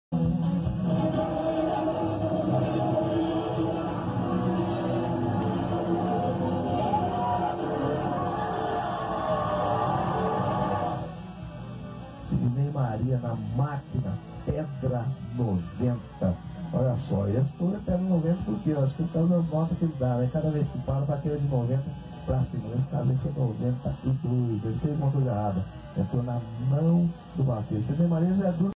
SIDNEY MARIA (EM MEMÓRIA) X PEDRA 90 - RODEIO DE JAGUARIÚNA 1999
Sidney Maria (em memória) montando o Touro Pedra 90 da Cia de Rodeio do Paulo Emilio no Rodeio de Jaguariúna - SP no ano de 1999
Campeão do Rodeio 🏆
Imagens: Canção Nova 📽